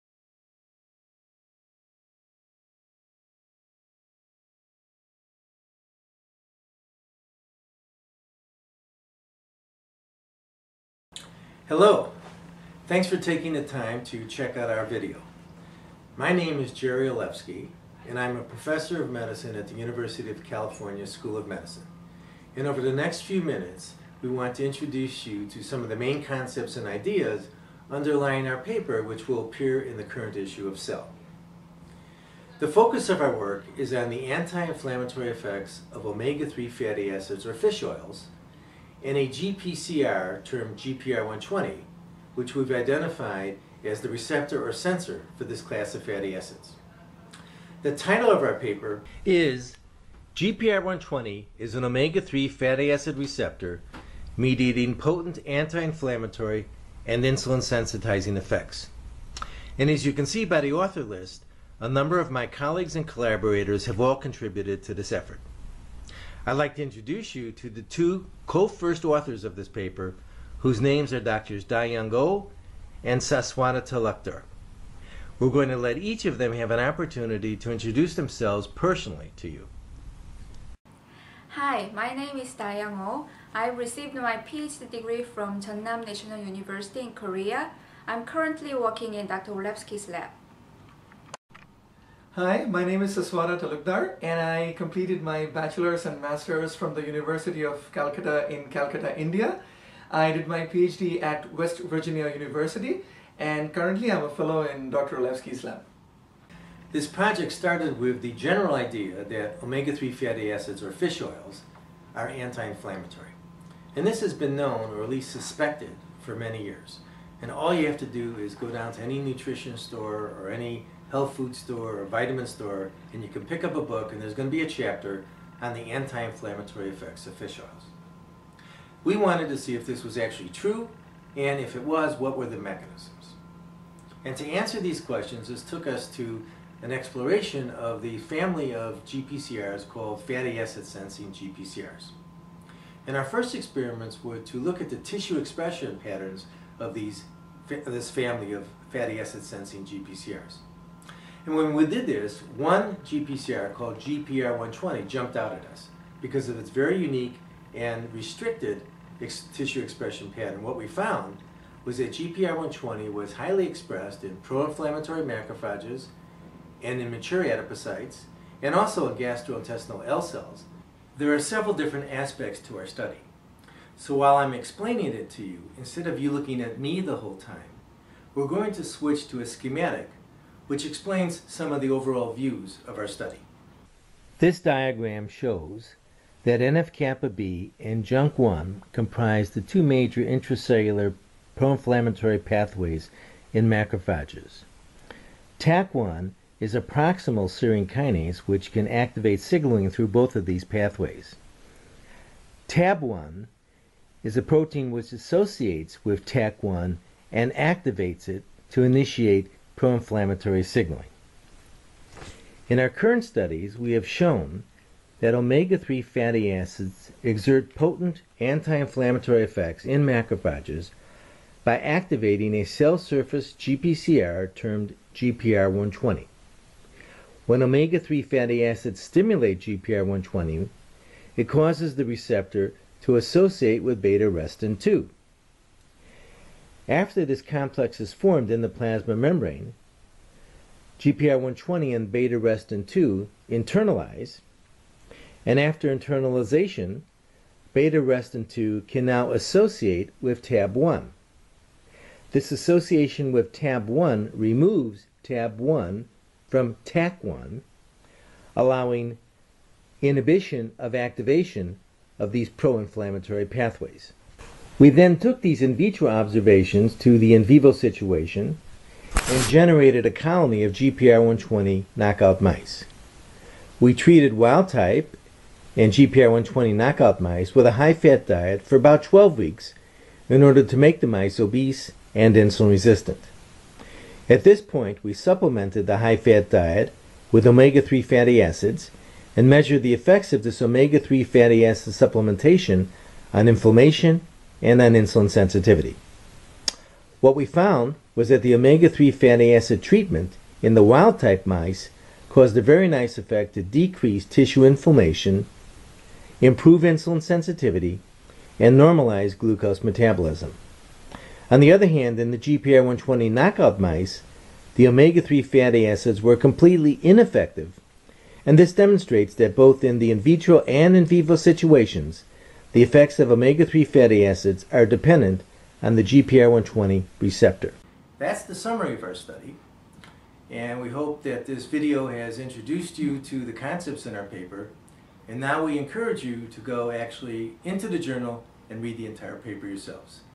Oiling the Immune System for Top Performance
Omega-3 fatty acids are a popular diet supplement, but the mechanisms underlying their beneficial properties have been mysterious. Watch and listen as Jerrold Olefsky takes you on a brief, personally guided tour of his lab's discovery of how omega-3 fatty acids in fish oil squelch inflammation and protect obese mice from diabetes.